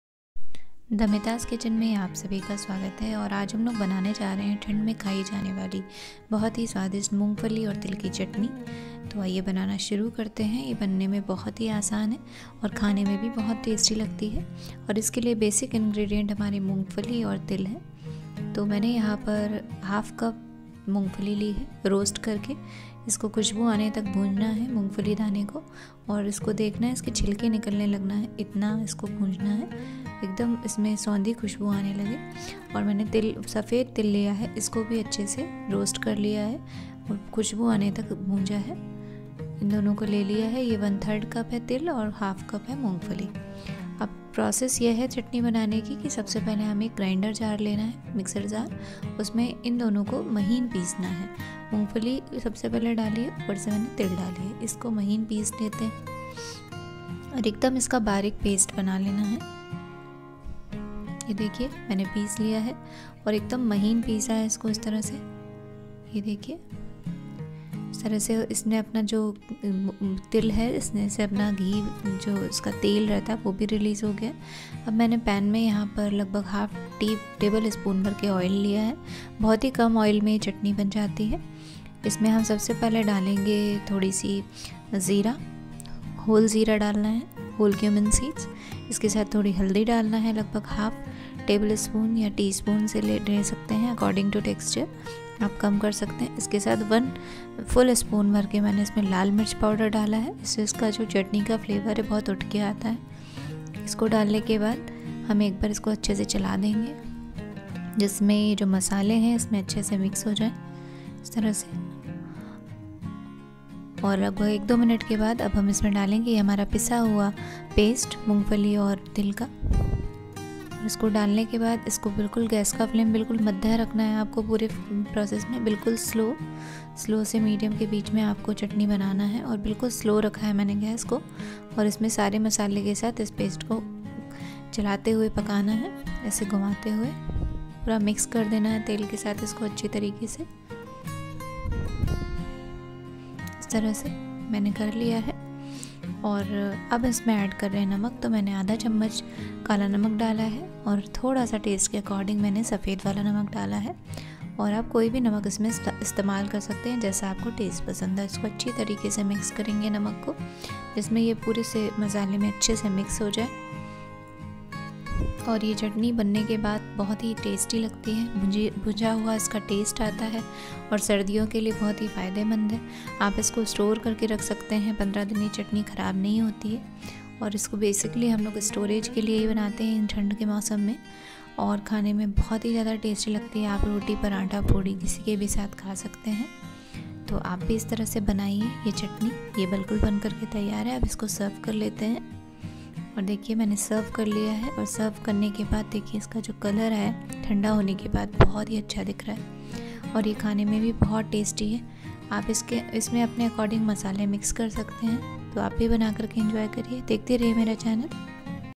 मूंगफली तिल चटनी || Groundnuts, Sesame Seeds Chatni || Chatni Chatpati
Ingredients:
 मूंगफली तिल चटनीpp
Groundnuts (मूंगफली ) -1/2cup
Sesame Seeds( तिल) -1/3cup
Salt ( namak)
Black Salt( kala namak)
Cooking oil-1/2 table spoon
whole cumin seeds -jeera
turmeric ( haldi) -1/2 teaspoon
red chilli powder ( lal mirchi) -1 table spoon.